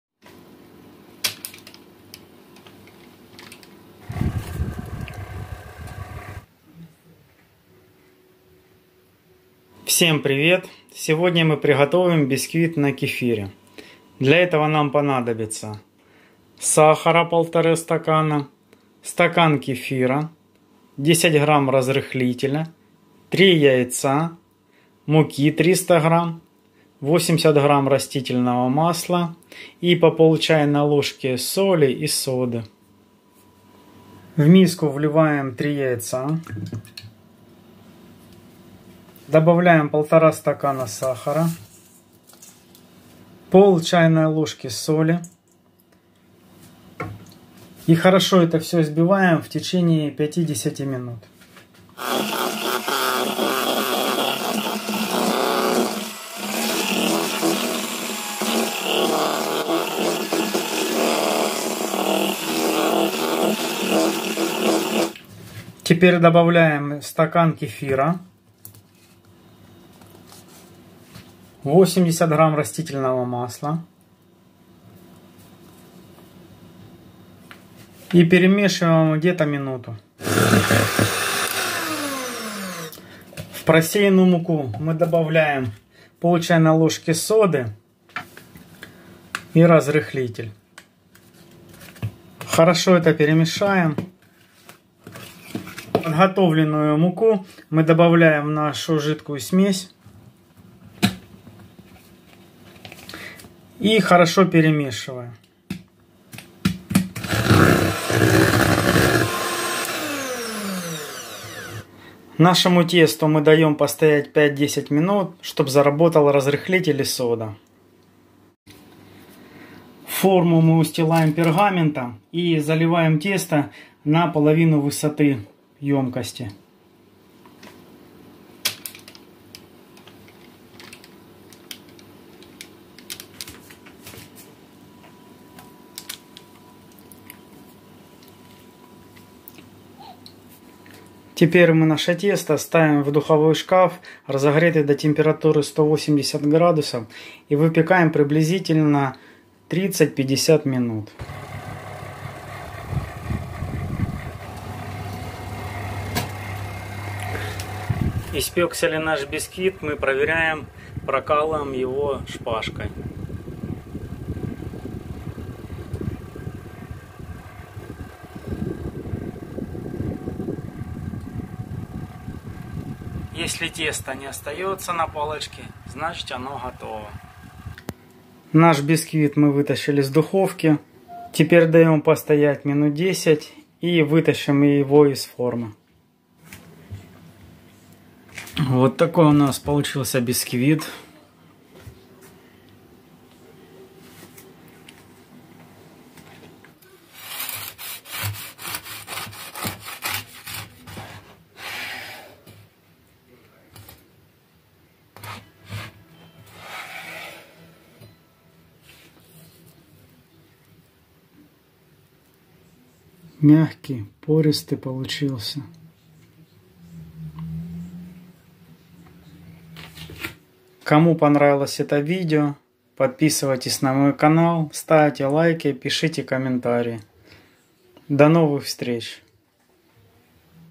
Пирог бисквитный на кефире.Мягкий,пушистый,влажный.
ПИРОГ НА КЕФИРЕ.
РЕЦЕПТ:
0,5 ч.ложки соли
0,5 ч.ложки соды
Разрыхлитель 10 грамм
Кефир 1 стакан
Растительное масло 80 грамм.